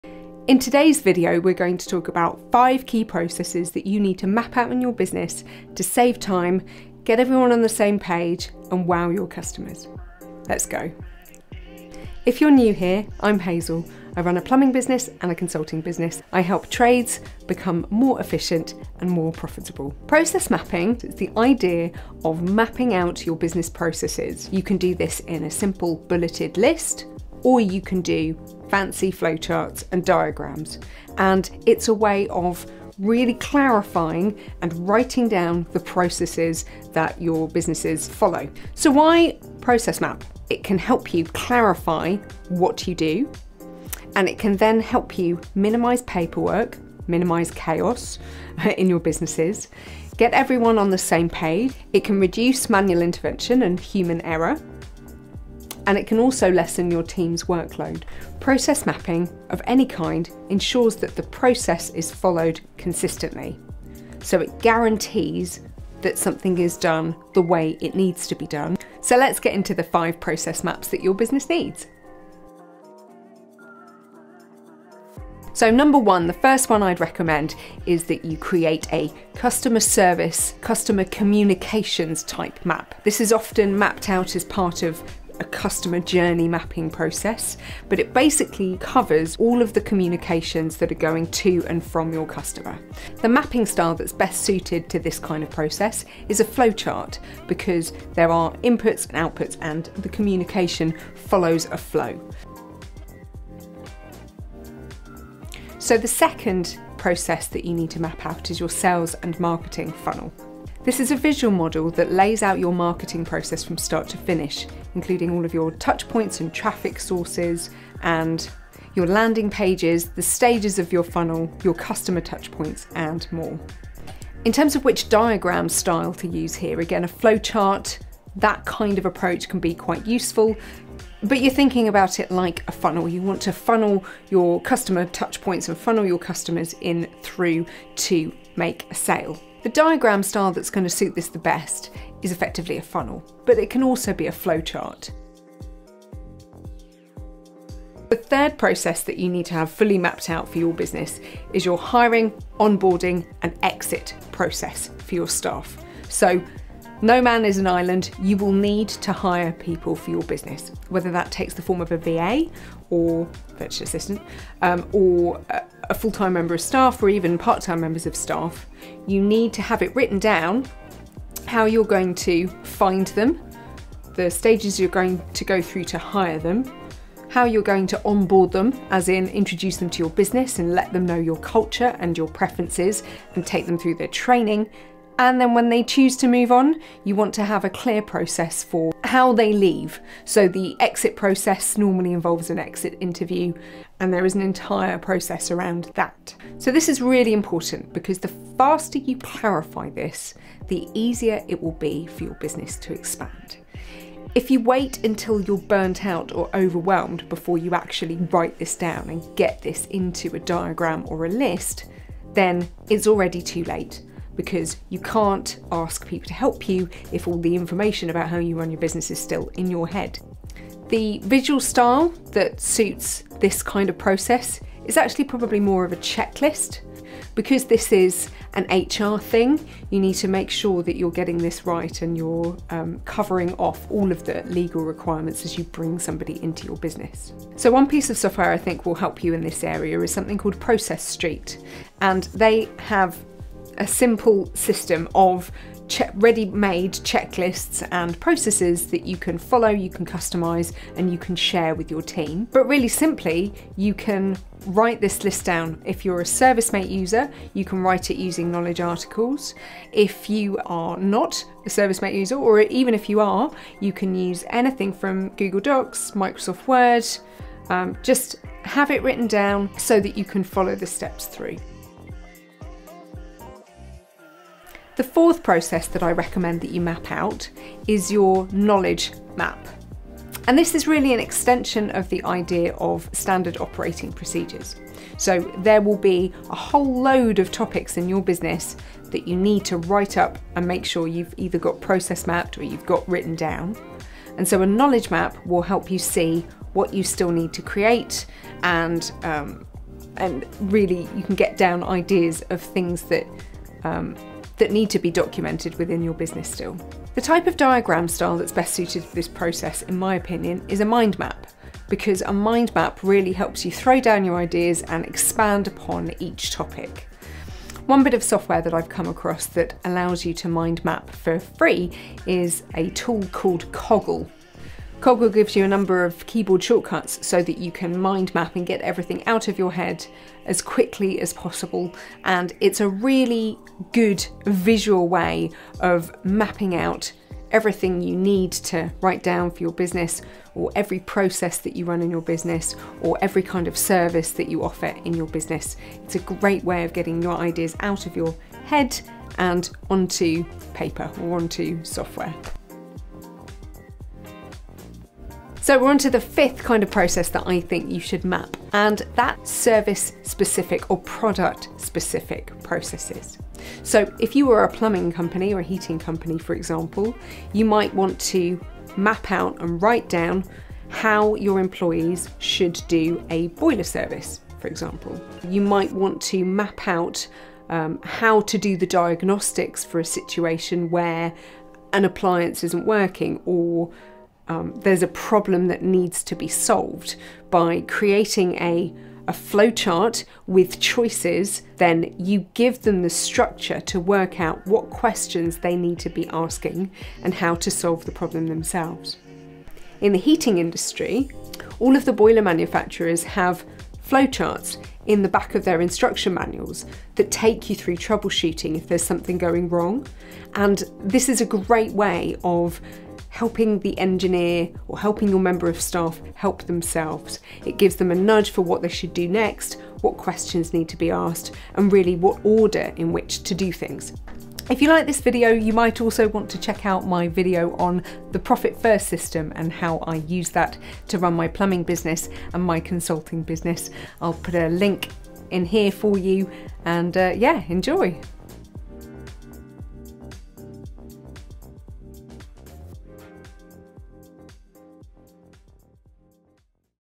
5 business processes you need to map out and the tools that make it easy
In this video, I summarise the 5 key processes you need to map out in your business and the most suited types of diagram and tools for doing so.

TIMESTAMPS
00:00 Intro
00:20 What is process mapping
00:38 Why map your processes
01:12 Customer communications
01:42 Sales and Marketing Funnels
02:25 Hiring, Onboarding & Exits
04:31 Knowledge Mapping
05:52 Service / Product Specific

ABOUT ME
I'm Hazel, a ServiceM8 expert and Notion lover who helps plumbers, electricians and other service & trade businesses get rid of paperwork and become more efficient 👩‍💻